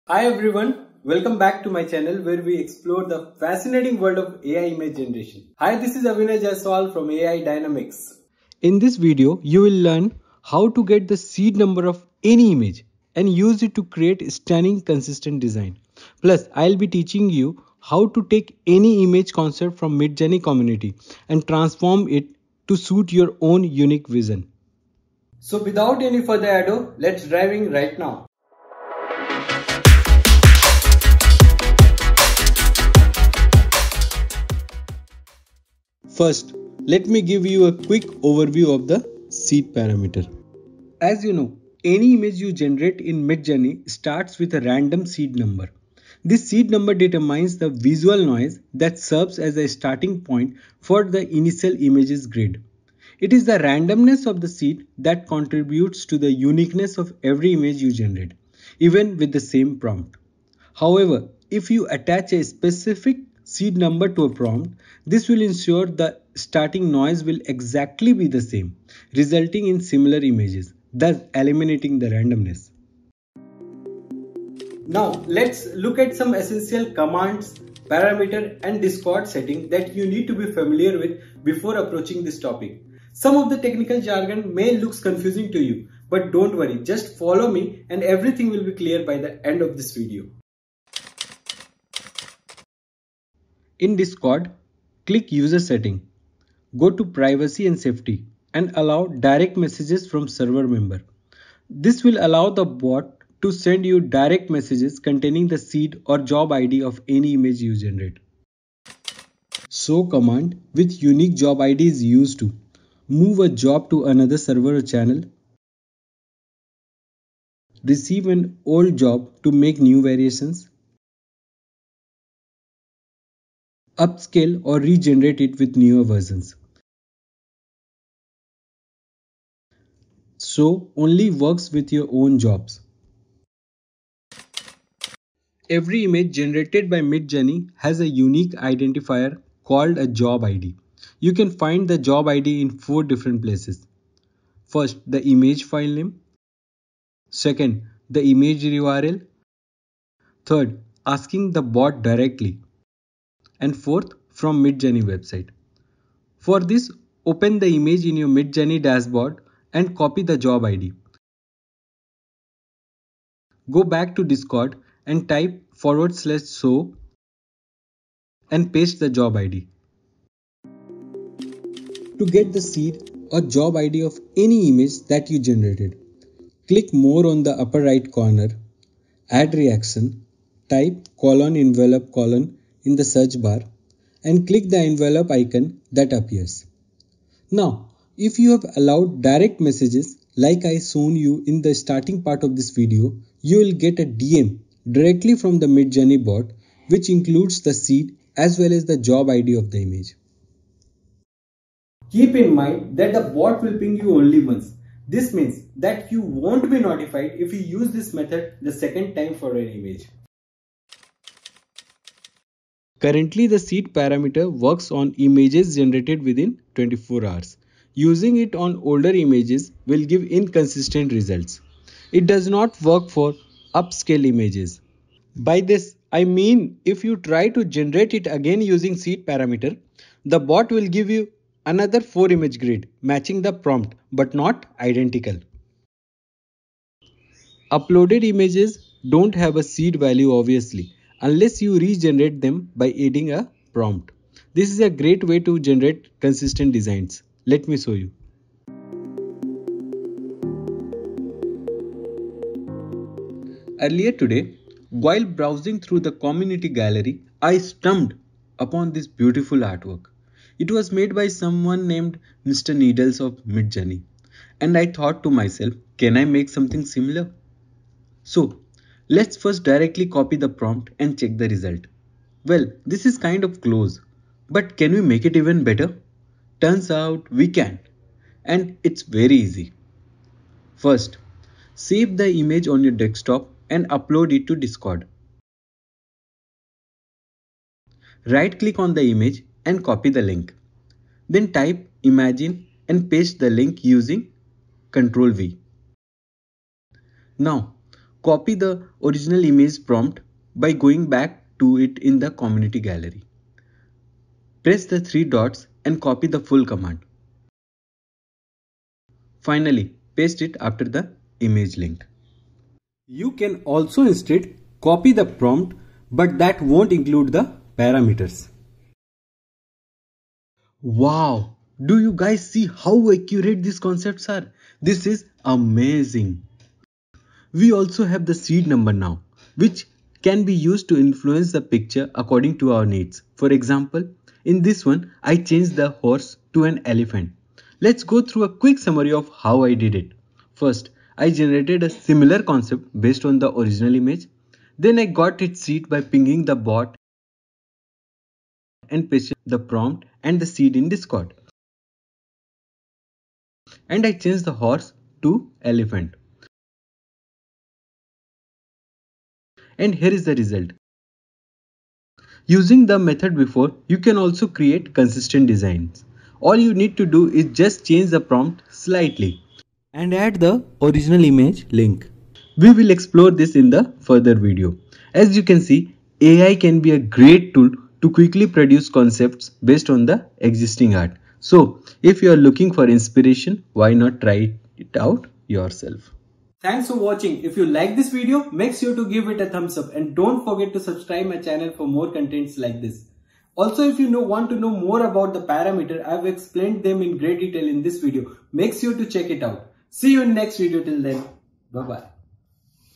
Midjourney Ai Tutorial: Replicate an Image & Discover Its --SEED & JOB ID
In this Midjourney Ai Tutorial I have explained how to find the --SEED number and JOB ID of your image, I have also explained how to copy the concept of any image in Midjourney
Community and use it to create consistent designs that match your unique vision.
0:00 Introduction
0:38 Overview of --Seed Parameter
1:17 Important Commands, Parameters, discord settings
2:53 Finding --seed & job id
4:12 Replicate an image

Here is the link to advanced Parameters in Midjourney